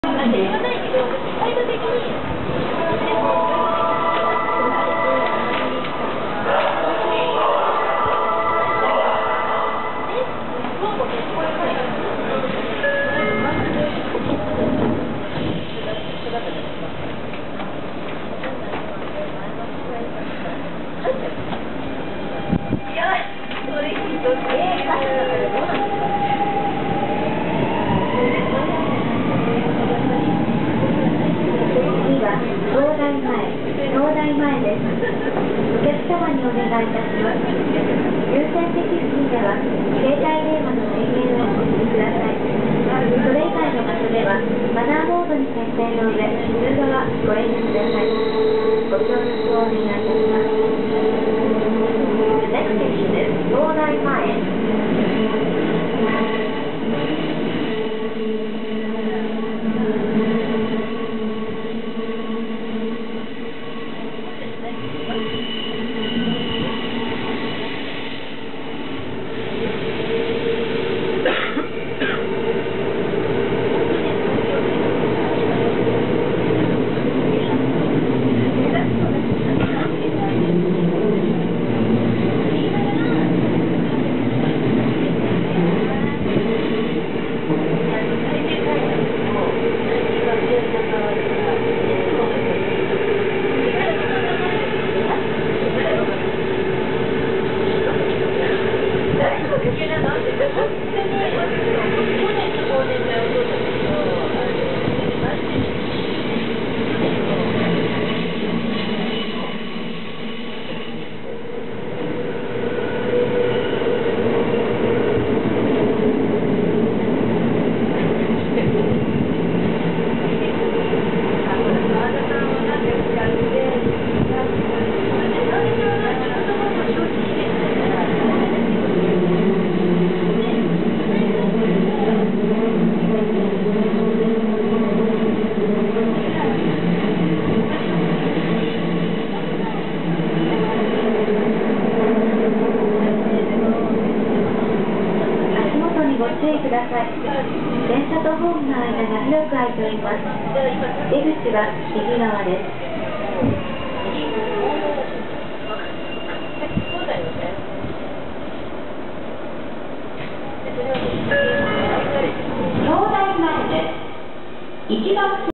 東京メトロ9000系試作車車内表示機(後楽園→東大前)
9001Fの1号車～3号車と6号車の車内表示機は他のタイプとは若干異なるタイプの車内表示機です。